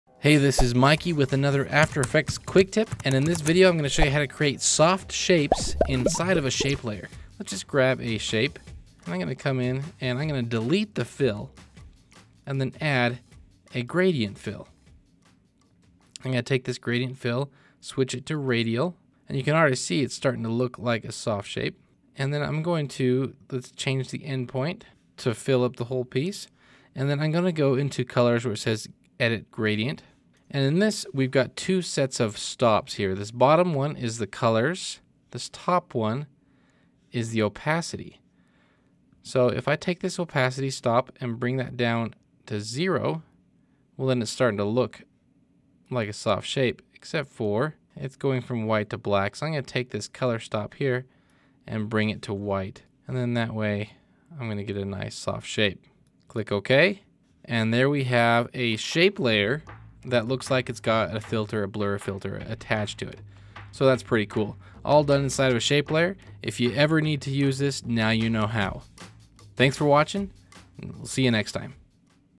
After Effect Quick Tip: shape layer transparency gradients
An after effects quick tip for shape layer people.  Create a soft shape with the gradient fill and clever transparency settings.  Check it out, its only 1 minute, it will probably take less time to watch the video than for me to explain it in this text and you read it.